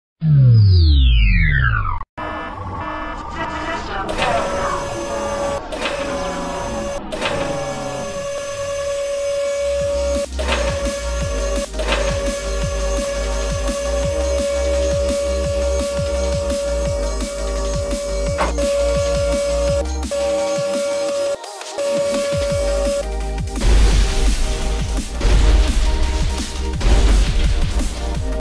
"Screw Head and his mysterious laboratory"
A futuristic robot is furiously typing away on his computer in his laboratory... He is determind... This robot is trying to power up his teleporter machine, unfortunately for himself though, the power goes out! Screw Head is stuck with no lights in his lab and no power to power all his technology!!! Screw Head must find a way to get the power back on in order to power up his teleporter so he can successfully travel in time.